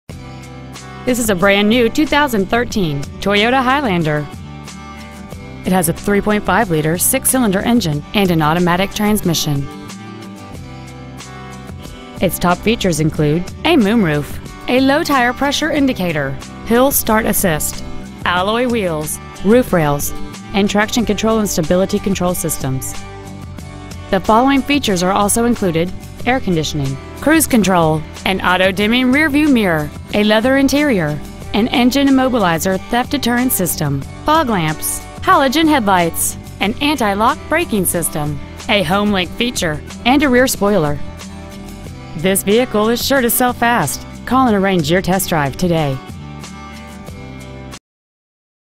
2013 Toyota Highlander Grapevine TX 76051
Year : 2013
 Make : Toyota
 Model : Highlander
 Engine : 3.5L DOHC 24-valve VVT-i V6 engine
 Trans . : Automatic
 Exterior : Gray
 Miles : 1
 Interior : -

 801 State Hwy. 114 East